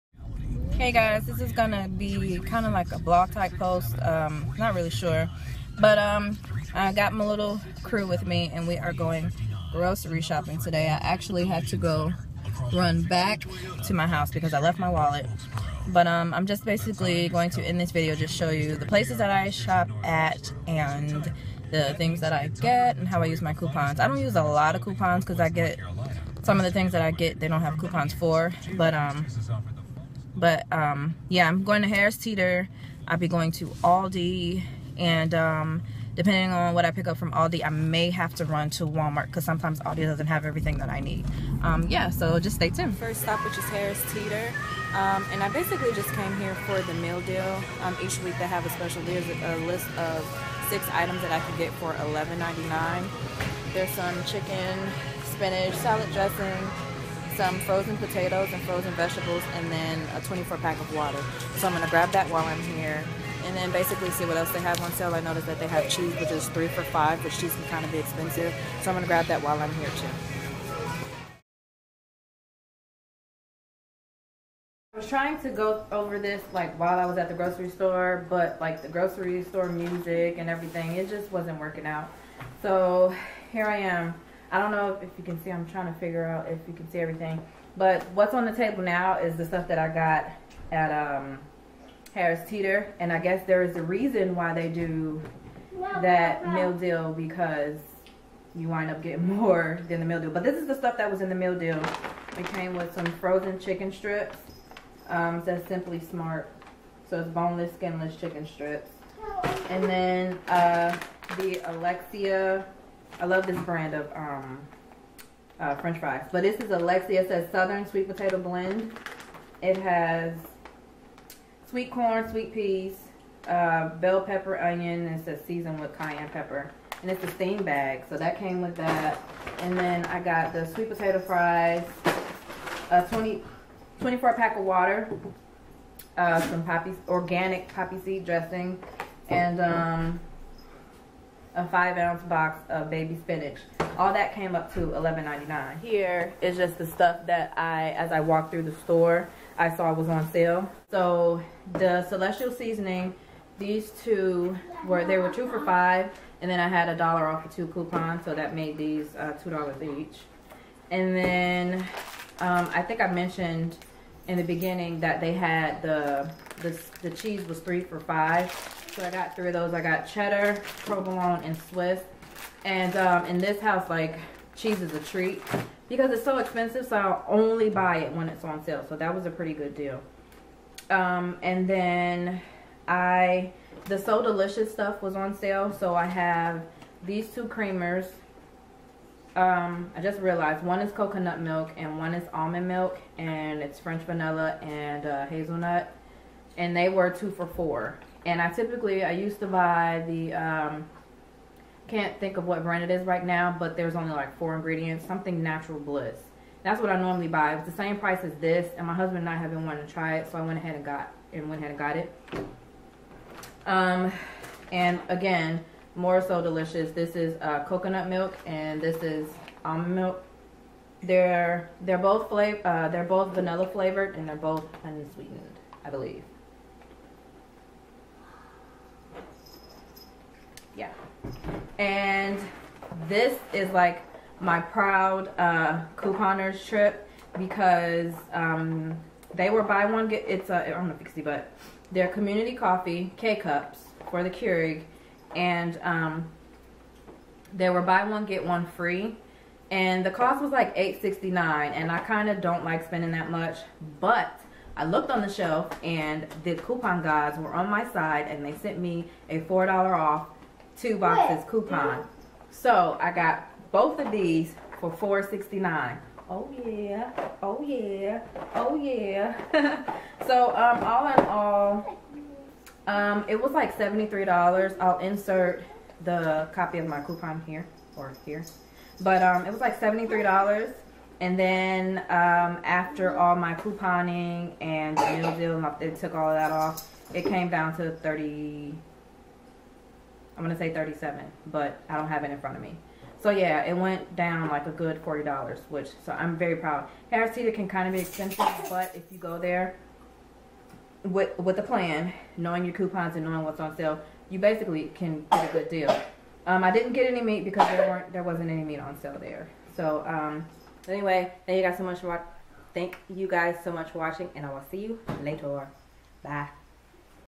Come Grocery Shopping With Me!
Just sharing my shopping experience at Harris Teeter with y'all!